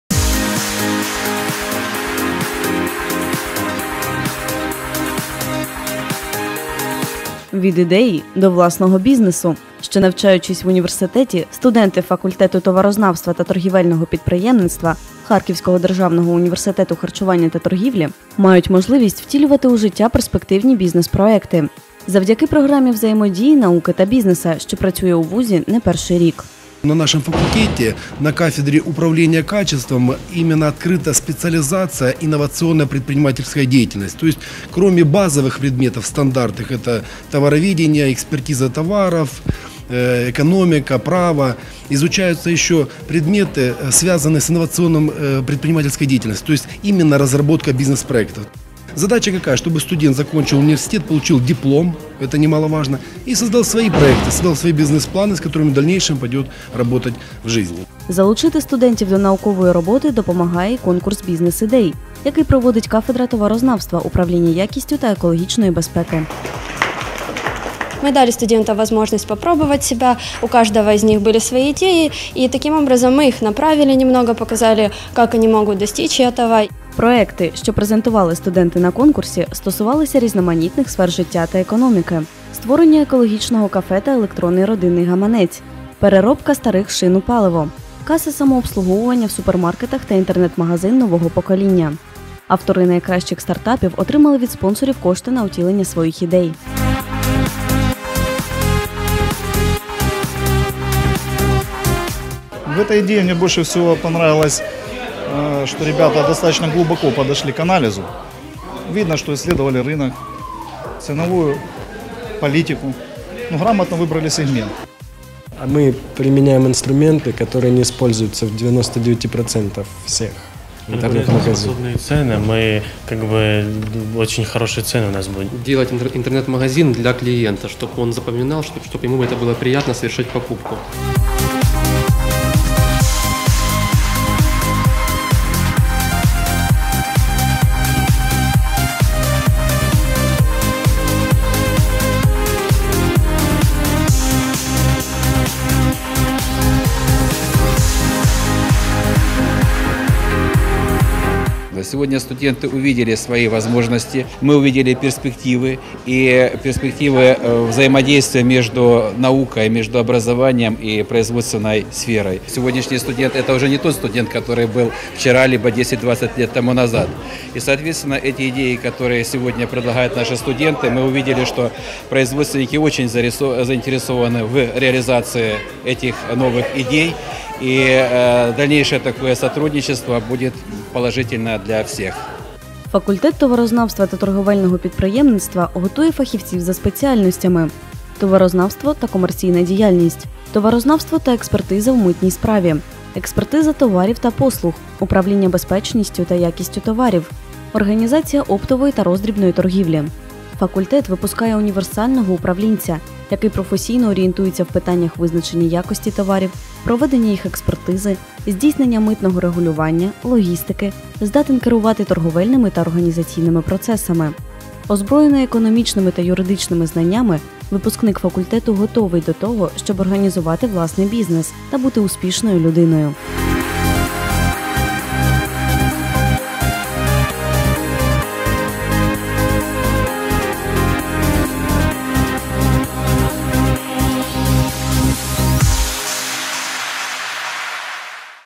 Факультет товароведения и торгового предпринимательства ХГУХТ. Конкурс бизнес идей.
Партнер показа Конкурс бизнес-идей на факультете товароведения и торгового предпринимательства Харьковского государственного университета питания и торговли. Еще во время учебы в университете студенты создают и воплощают в жизнь бизнес-проекты.